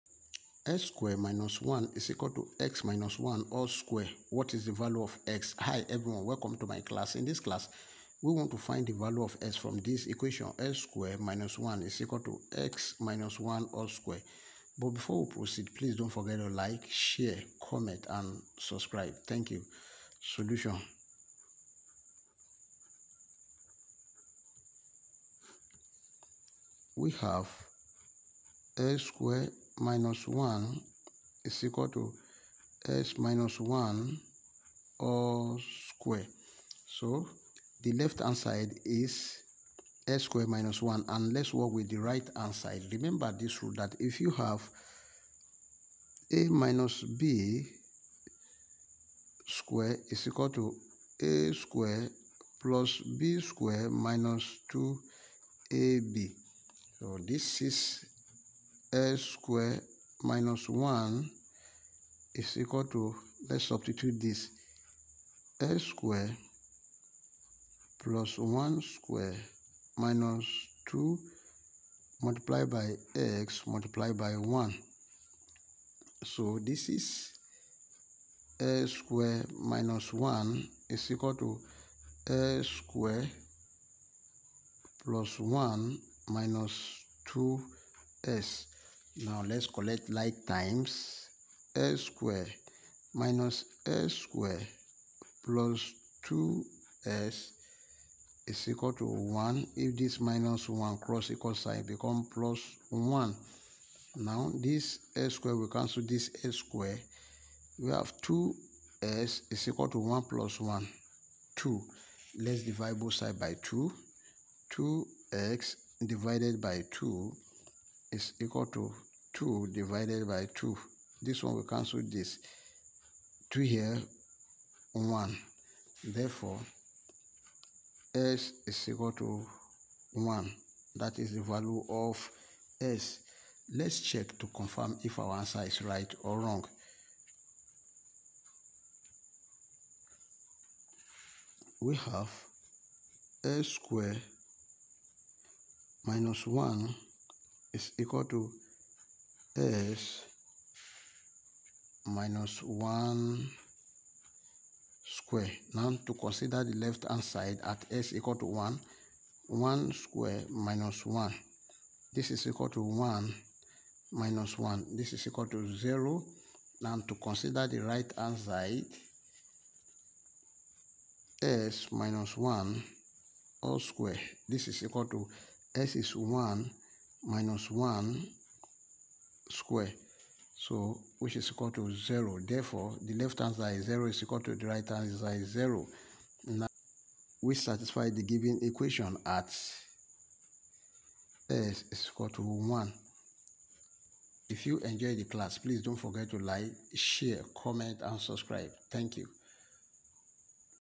Nice Algebra Problem x=?
In This video, you will learn how to calculate the given nice algebra problem very fast with this new trick.